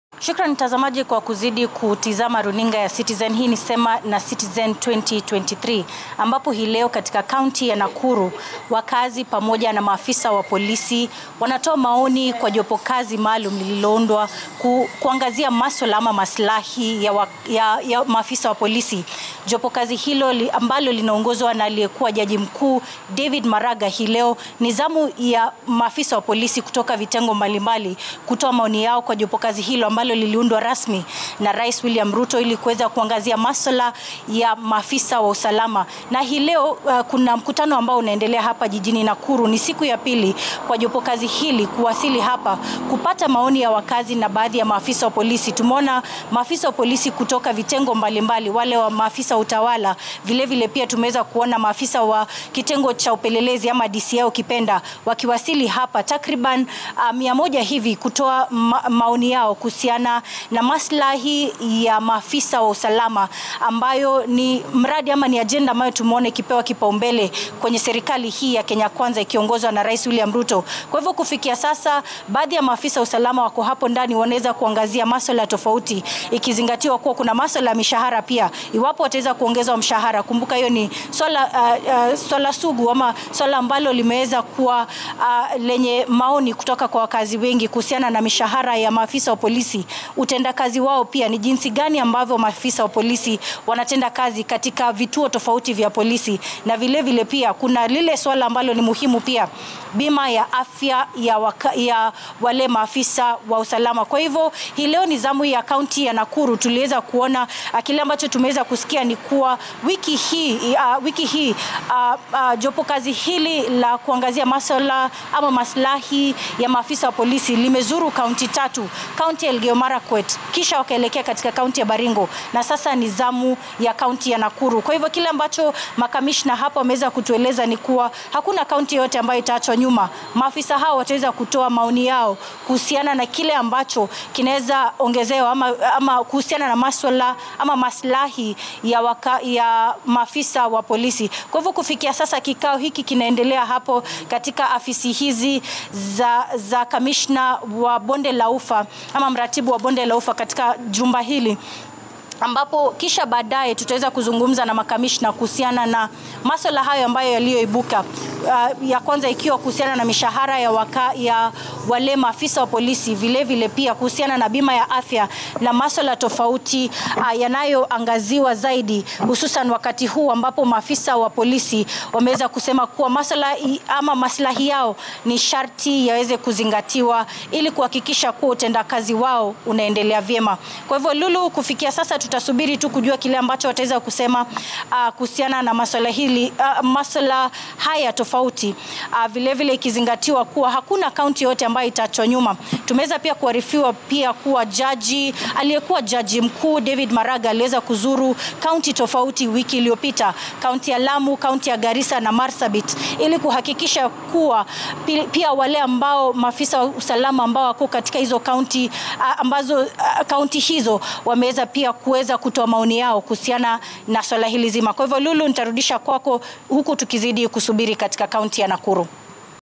Jopokazi ya David Maraga lazuru kaunti ya Nakuru
Wakaazi na maafisa wa polisi katika kaunti ya Nakuru wanatarajiwa kutoa maoni yao hii leo kwa Jopokazi maalum la kutathmini maslahi  ya maafisa wa polisi.Jopokazi hilo linaloongozwa na aliyekuwa jaji mkuu David Maraga linatarajiwa kuzuru kaunti tofauti kupata maoni kuhusu utendakazi wa  maafisa wa  polisi na jinsi idara hiyo itaboreshwa